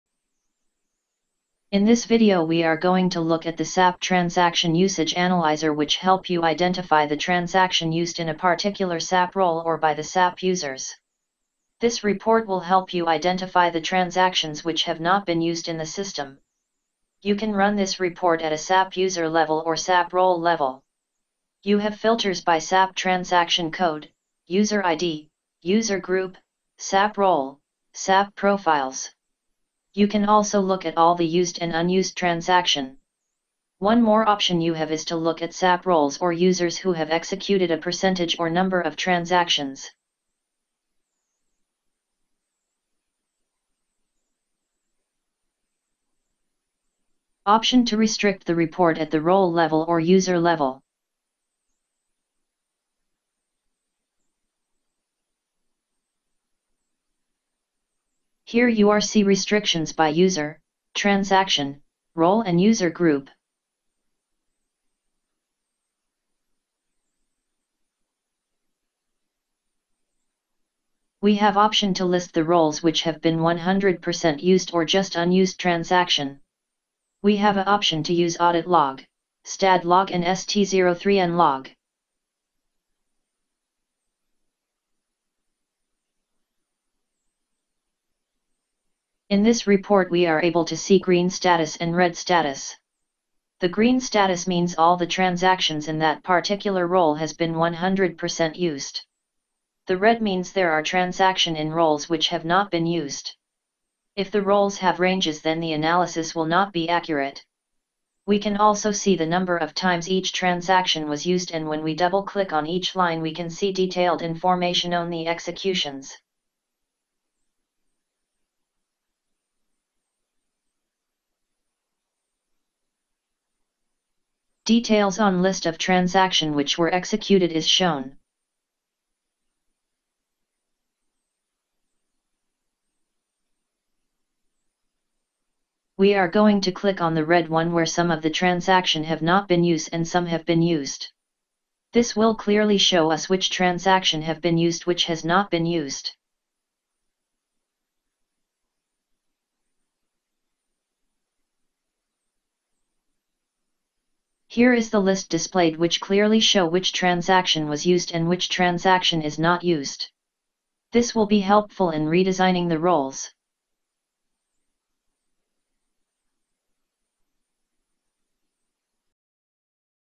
SAP Transaction Usage Analyzer- RBE Tool
If you want design your role based on usage you will need to how many transactions within the SAP role or SAP user has been really used. This will help you redesign the Role Efficiently.